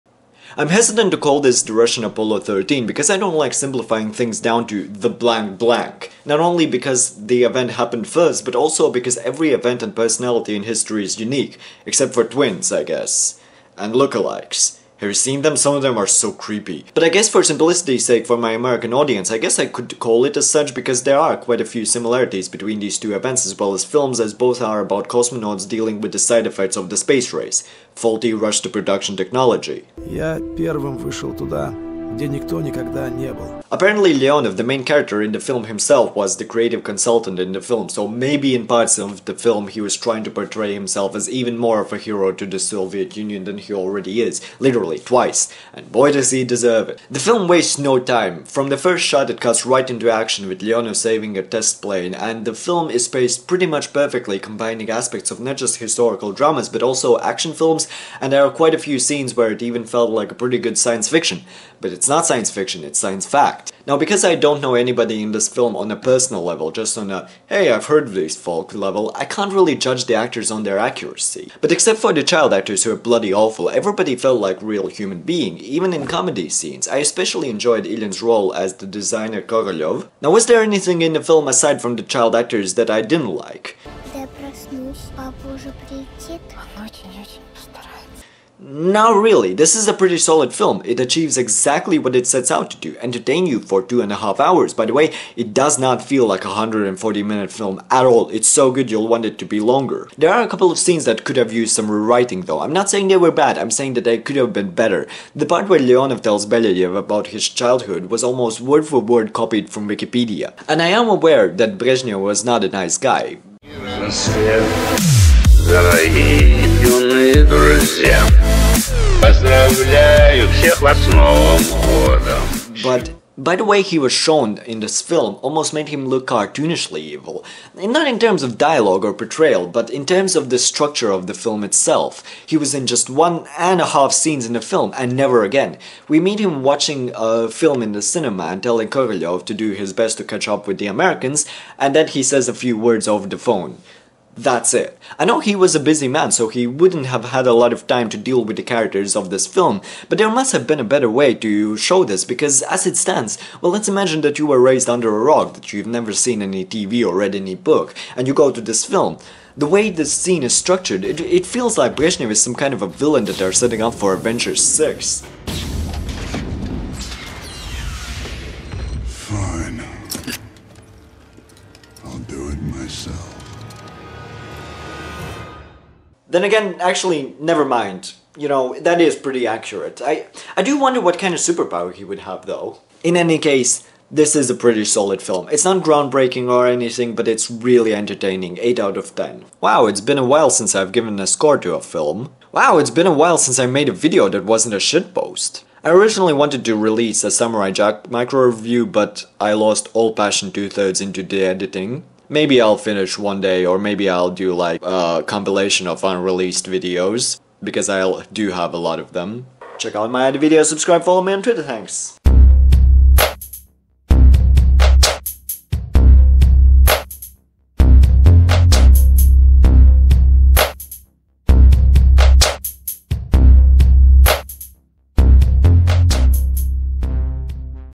Spacewalker Review
Review of Russian film "Spacewalker", AKA "First Time" or "Age of Pioneers " (Russian title: Время первых). Directed by Dmitriy Kiselev.